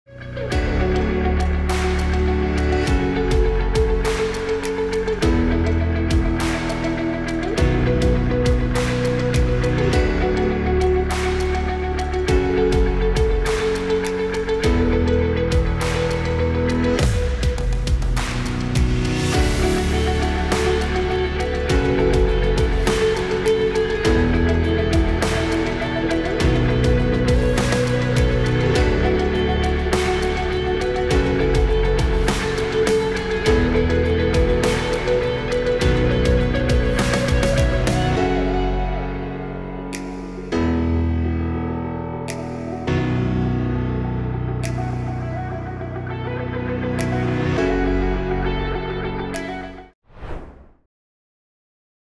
Narrowest House In The World!
This is the world’s narrowest house. A fully functional space that is just 4 feet wide. Would you want to stay in this house? Watch this Jist to know more about this tiny house.